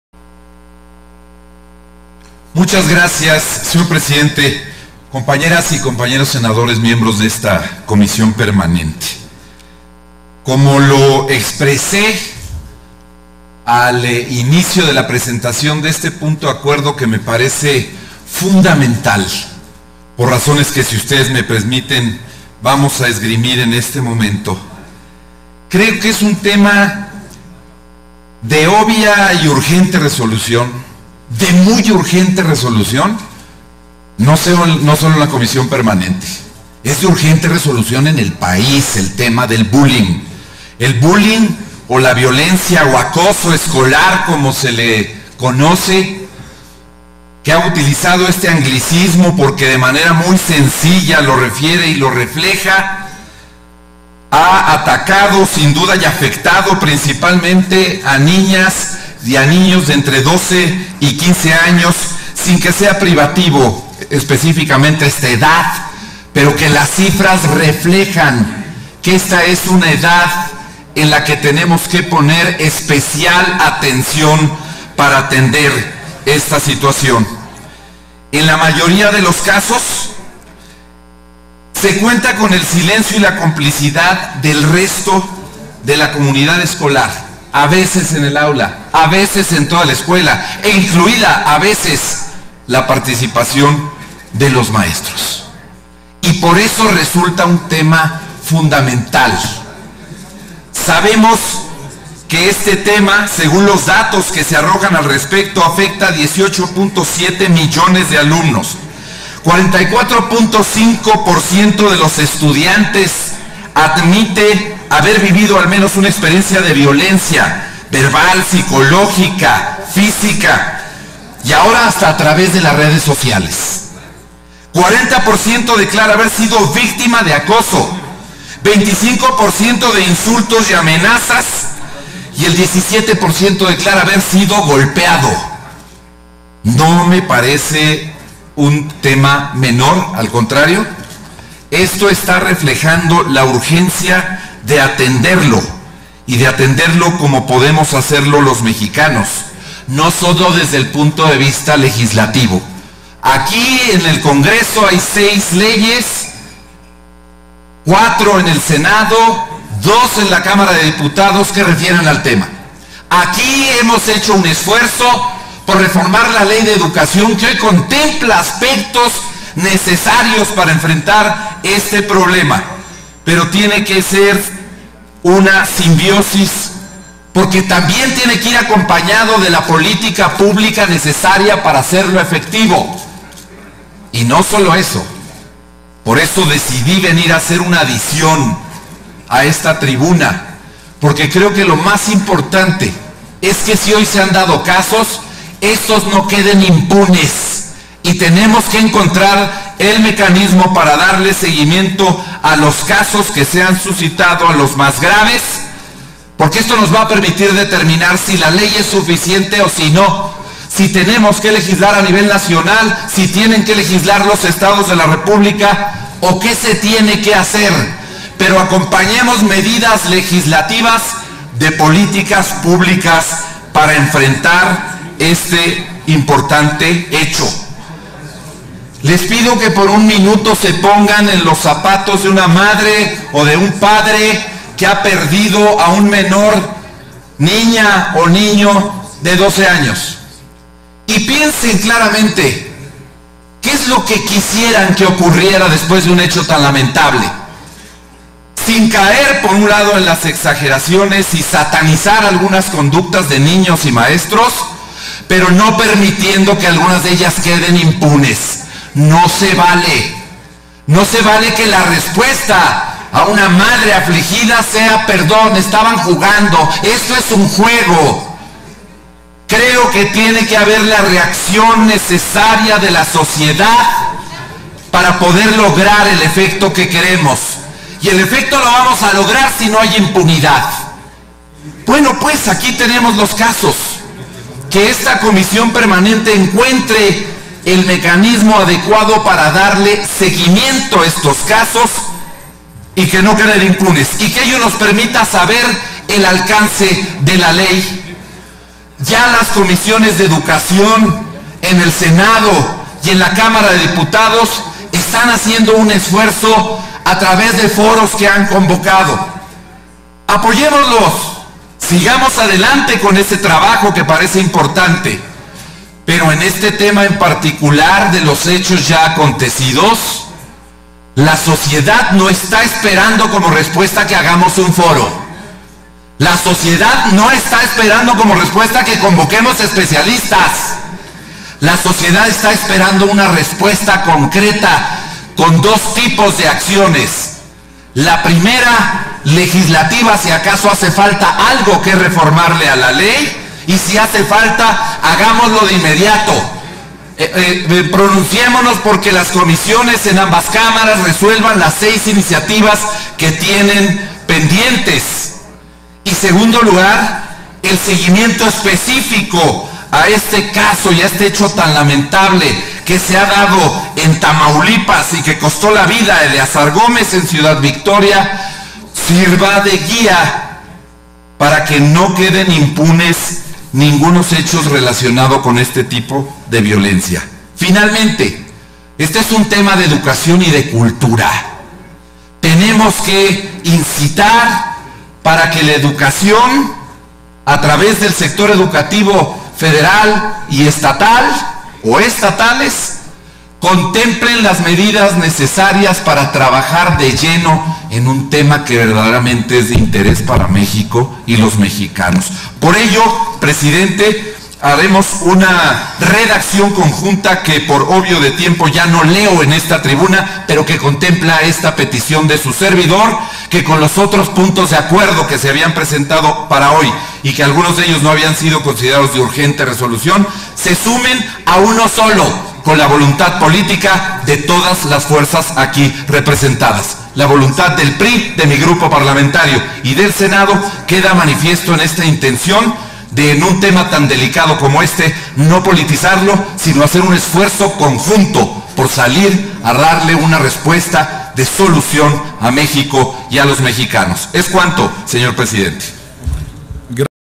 Terminar con la impunidad en la practica del Bullying
Participación en tribuna del Senador Omar Fayad en relación al bullying.